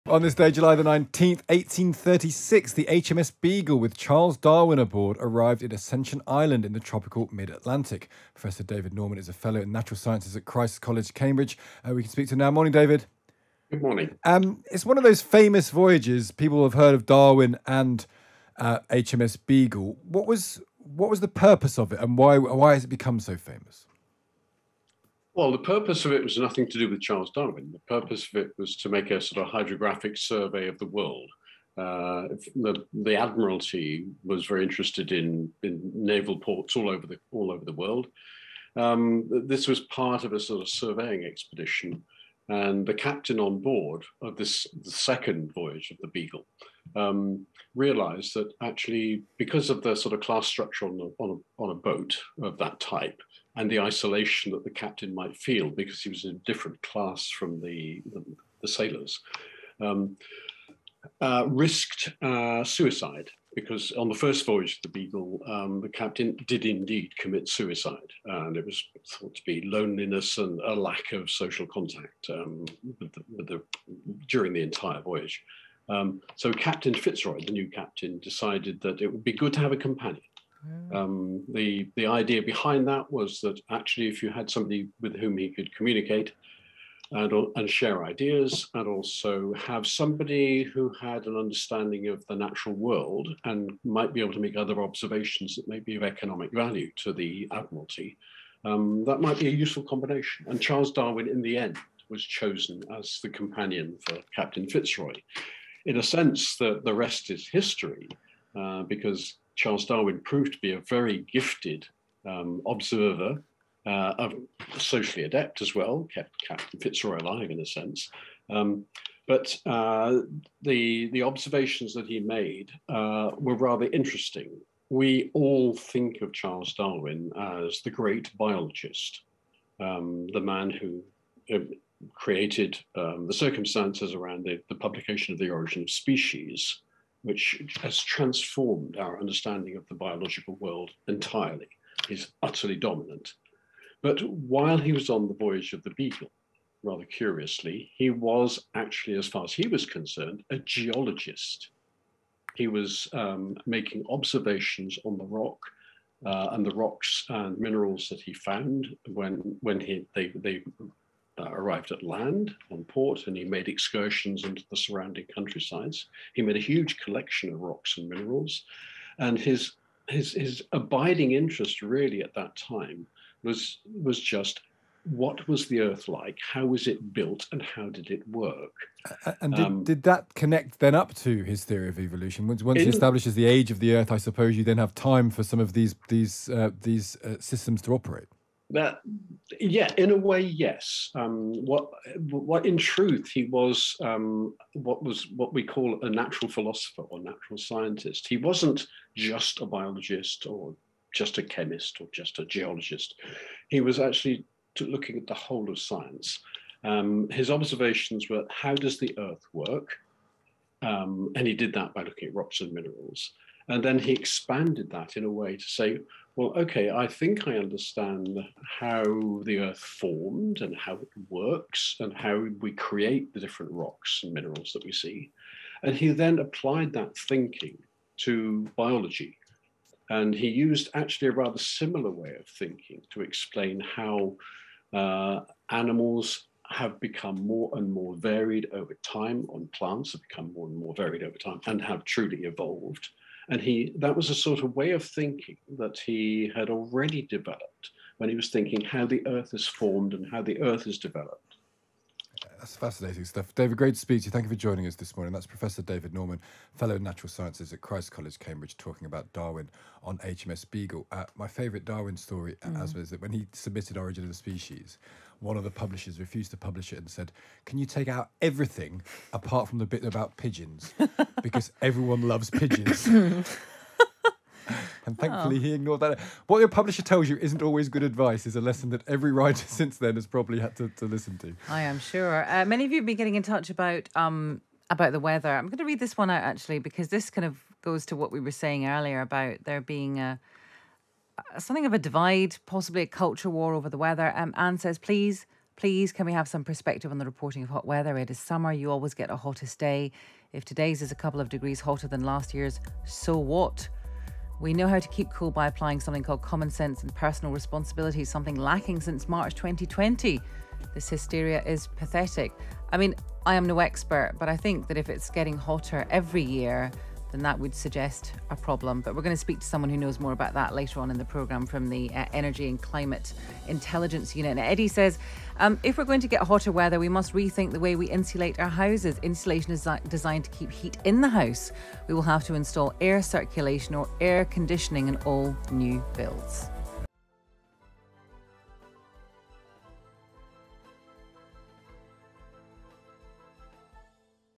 The significance of Darwin's voyage to Ascension Island | On This Day
On this day July 19th 1836 the HMS Beagle with Charles Darwin on board arrived in Ascension Island,  in the tropical mid-Atlantic.

We are joined by Professor David Norman, Fellow in the Natural Sciences at Christ’s College Cambridge.